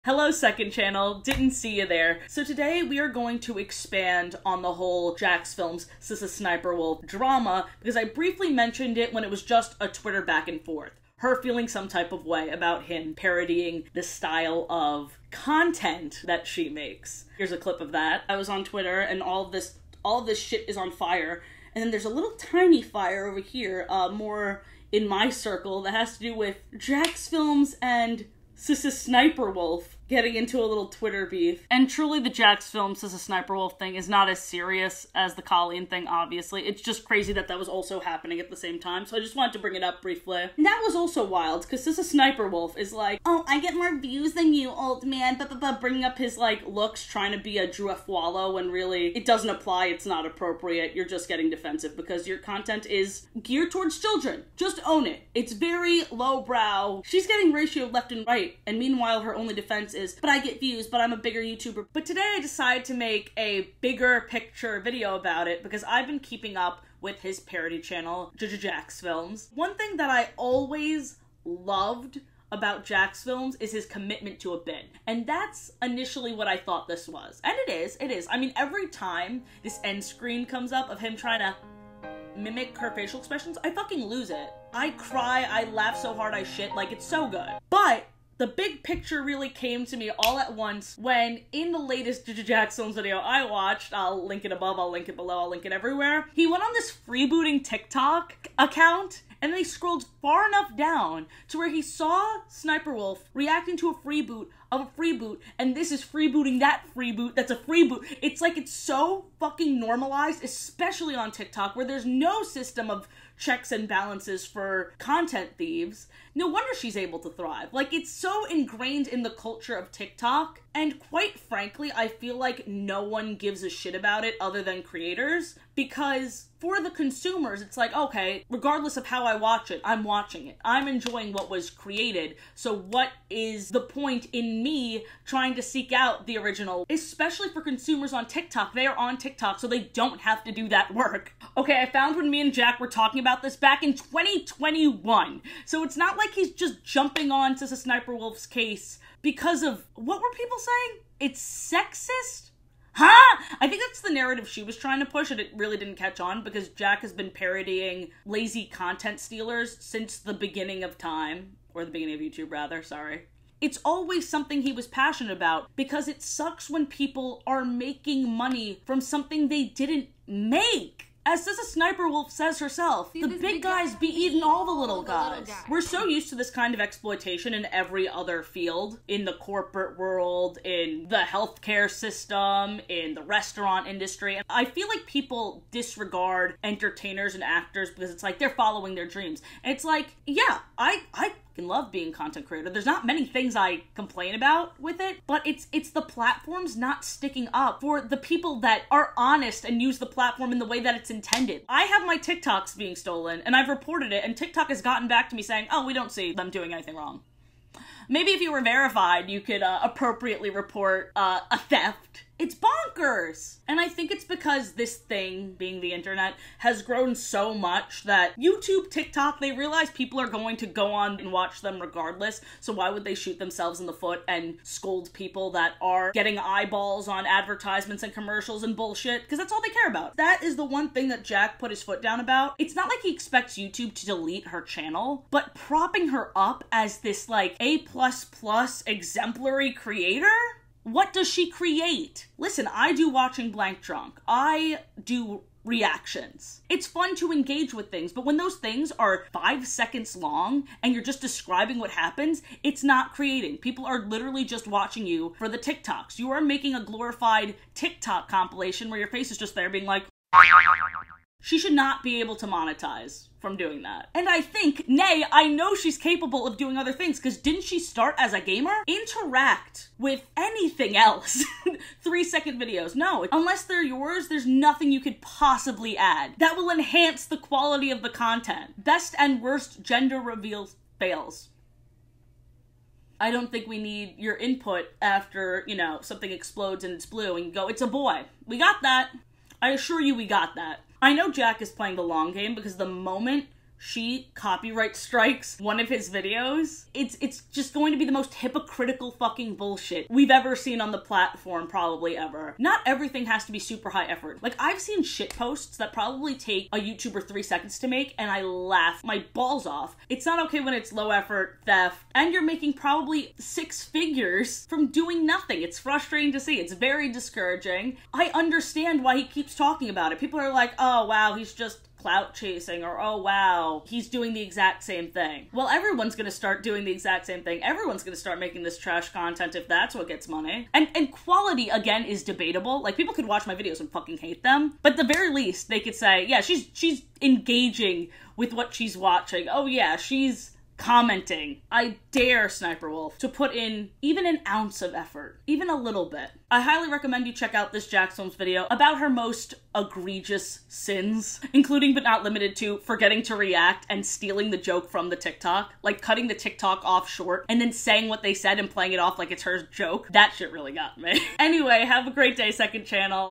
Why sssniperwolf should make you as mad as jacksfilms (youtube doesn't care about content theft)
she be eatin AAAALL the little guys!

Shoutout to @jacksfilms and @jjjacksfilms for calling out this exploitative behavior